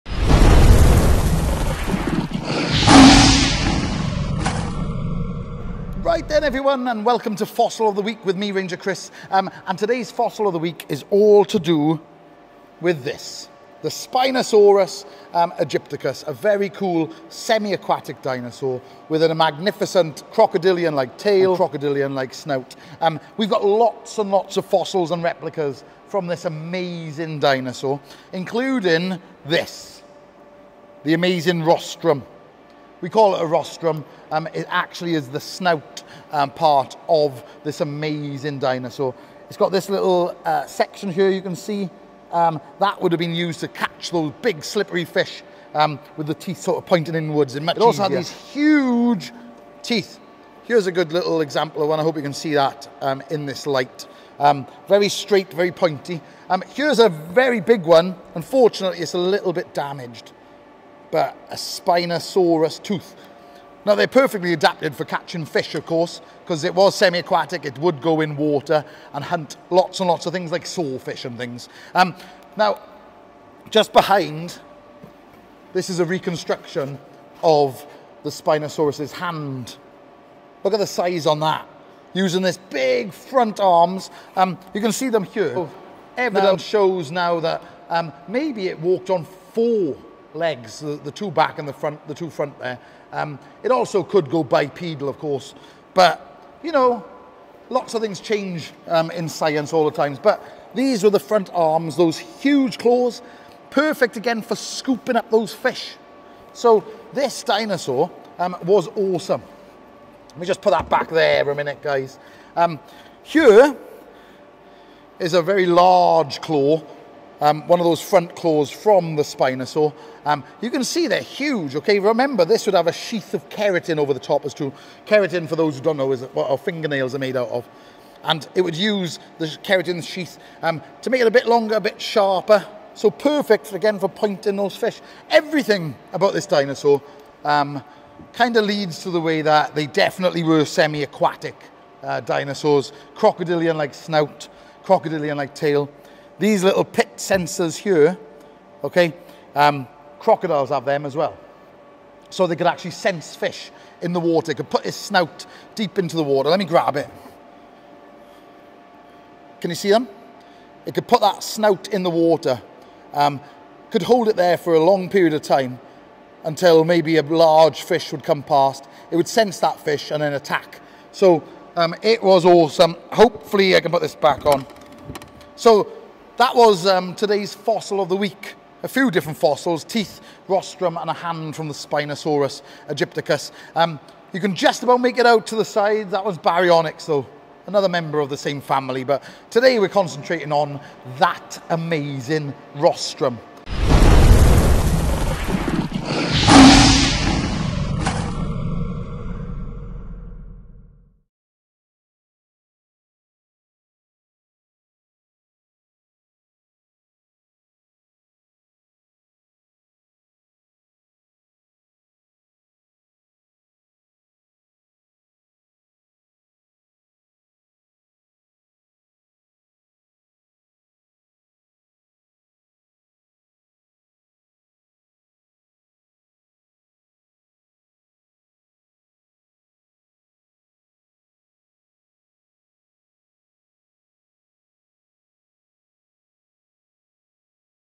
Fossil of the week. 🦖 Spinosaurus
Check out this amazing selection of Spinosaurus fossils and replicas.